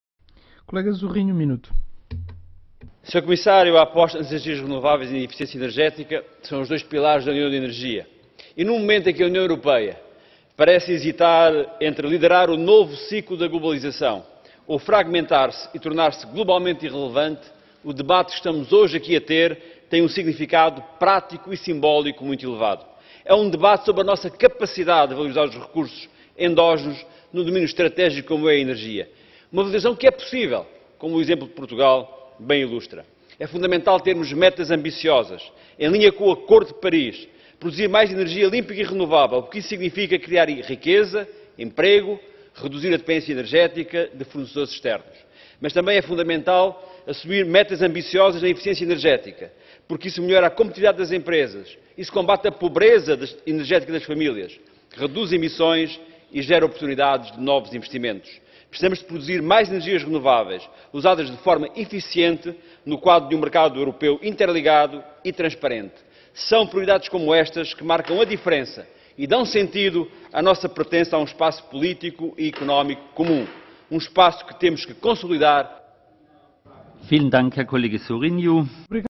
Carlos Zorrinho no debate sobre o relatório relativo aos progressos no domínio das energias renováve
Carlos Zorrinho no debate sobre o relatório relativo aos progressos no domínio das energias renováveis - Bruxelas - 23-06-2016.